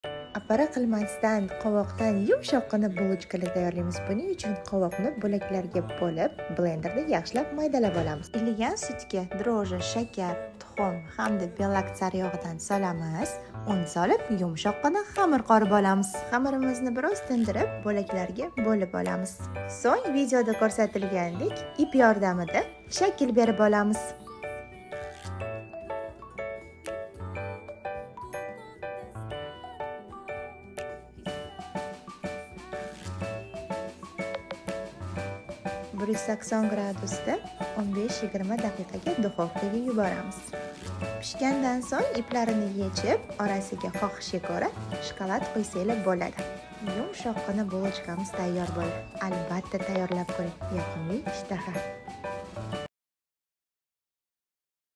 A bun.Bulochka tayyorlash😋tiktok
Delicious Pumpkin Buns

Necessary materials
200 ml of milk
1.osh.q drug
1.5 tablespoons of sugar
I used half of the pumpkin
50-60 gr Butter or margarine
1 egg
350-400 gr of flour
Optional chocolate

Mazzali qovoqli bulochka

Kerakli masaliqlar
200 ml sut
1.osh.q droje
1.5 osh.q shakar
Qovoq man yarmini ishlatdim
50-60 gr Sariyog’ yoki margarin
1 ta tuxum
350-400 gr un
Xoxishga ko’ra nachinkaga shokolad